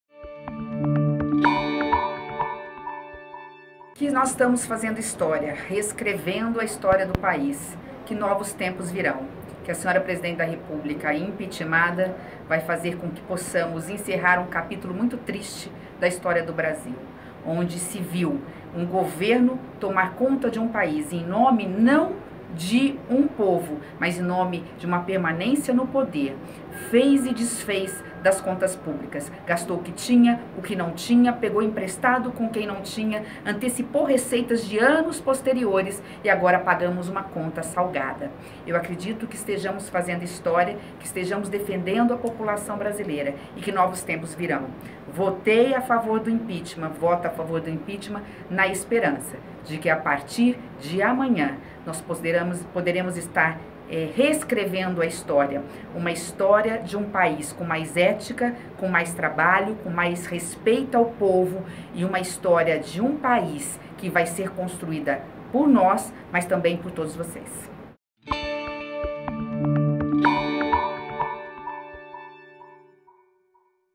Simone Tebet: 'Estamos reescrevendo a história do país'
A senadora Simone Tebet (PMDB-MS) disse que nesta quarta-feira (31), com a votação final do julgamento de Dilma Rousseff, "encerra-se um capítulo muito triste da história do Brasil, onde se viu um governo tomar conta do país não em nome do povo, mas em nome da permanência no poder". Simone disse que votará a favor do impeachment "na esperança de que a partir de amanhã nós poderemos estar reescrevendo a história de um país com mais ética, mais trabalho, mais respeito ao povo".